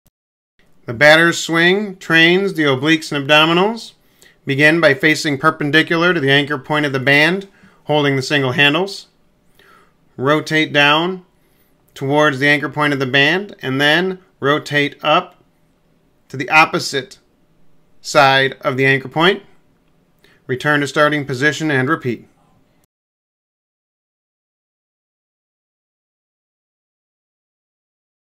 STOP Wasting Your Time with Ineffective Power & Core Workouts!
The Fitness channel for beginners and pros. Athletic training using the Batter Swing exercise with single-handle Resistance Bands is an excellent workout for improving rotational strength and power in the core and upper body. This dynamic movement mimics a baseball swing, targeting the obliques, shoulders, and arms for enhanced muscle definition and functional strength. Resistance Bands offer adjustable progressive resistance and a secure, ergonomic grip, ensuring optimal form and control throughout the swing. Incorporate the Batter Swing into your fitness routine to boost core power, improve upper body strength, and achieve a more explosive, well-toned physique. This dynamic exercise targets your obliques, shoulders, and core for a more powerful swing. Whether you're a beginner or an experienced athlete, resistance bands are perfect for adding extra tension and improving your form. Don't miss out on this unique exercise to boost your explosive power and elevate your training routine!